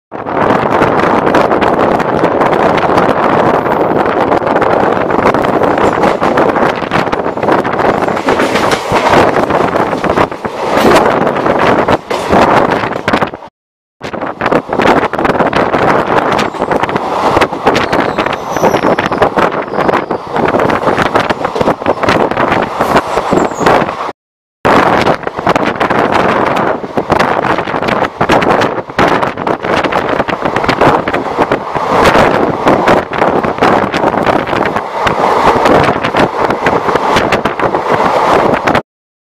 Chennai Suburban Railways
Suburban railways from Beach to Tambaram Spotting
This video was taken using Sony cybershot